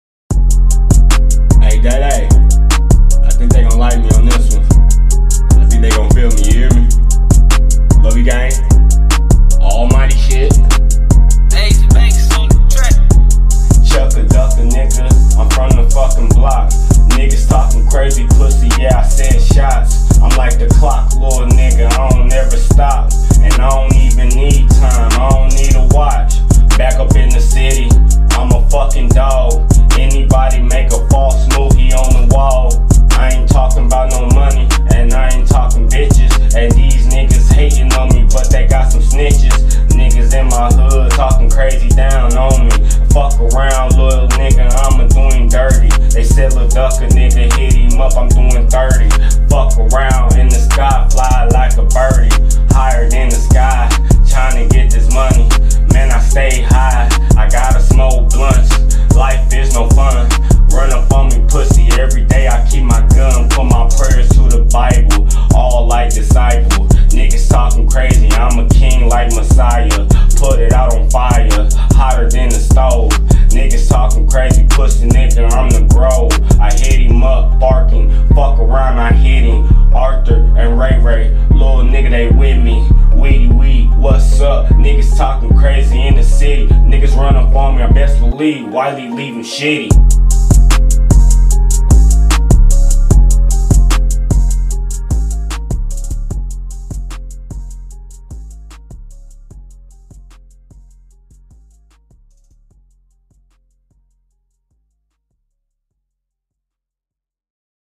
Notice By (ChuckaDucka)
This Explains Whats Goes On In Muncie, Indiana And Give Fans A Chance To Know What My Life About Or Whats Going On .